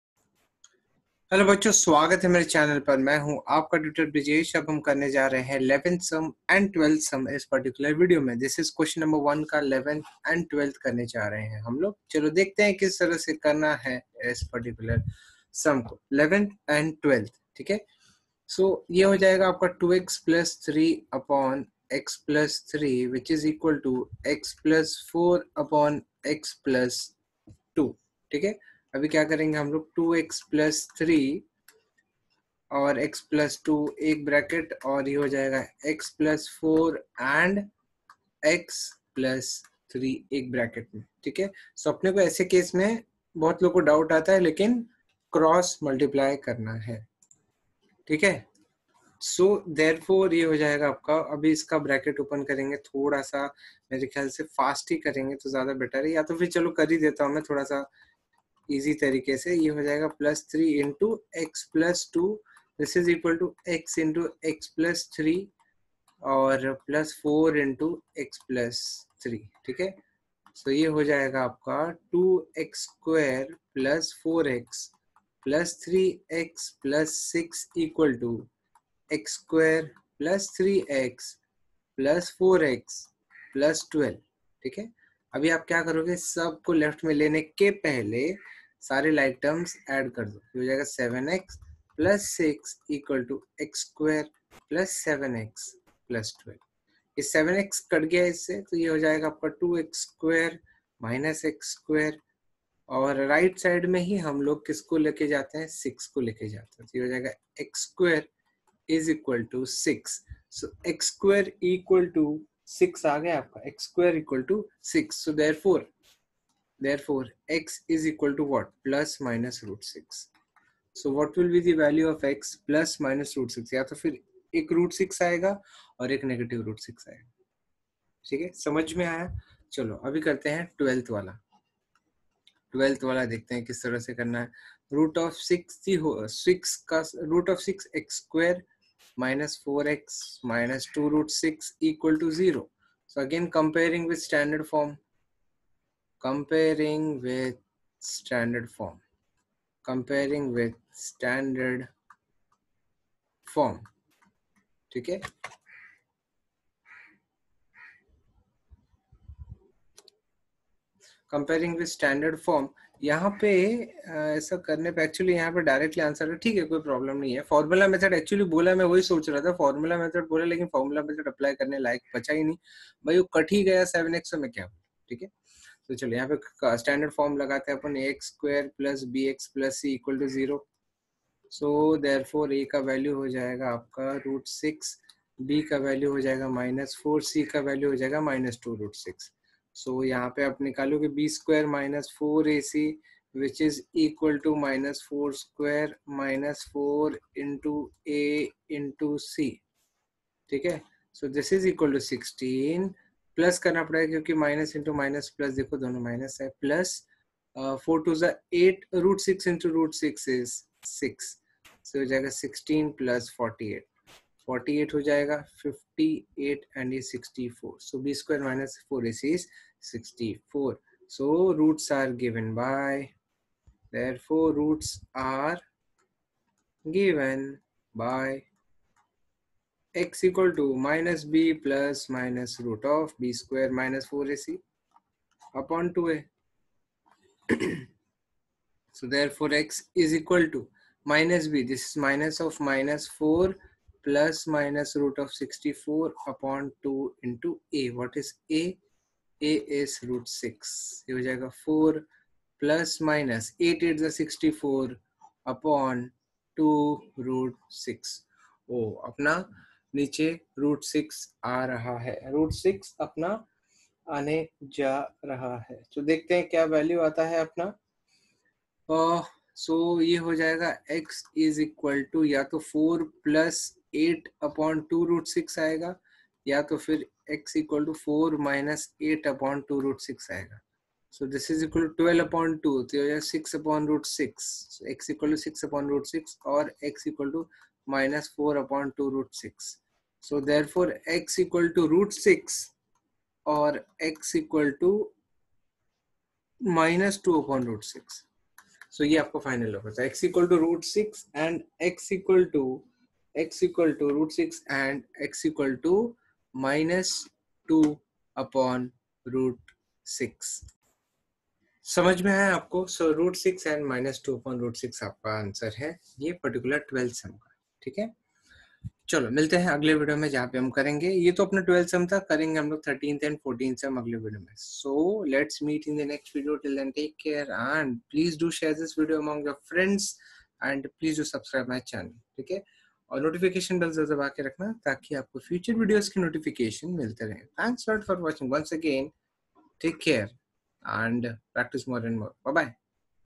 #39 | ICSE | Class X |Chap - QUADRATIC EQUATIONS (Full Chapter) | Selina | Exercise 5D- 1(xi, xii)
QUADRATIC EQUATIONS | Selina | Exercise 5D- 1(xi, xii)
1. Solve, each of the following equations, using the formula:
(xi) 2x + 3/ x + 3 = x + 4/ x + 2
(xiii) 2x/ x – 4 + (2x – 5)/(x – 3) =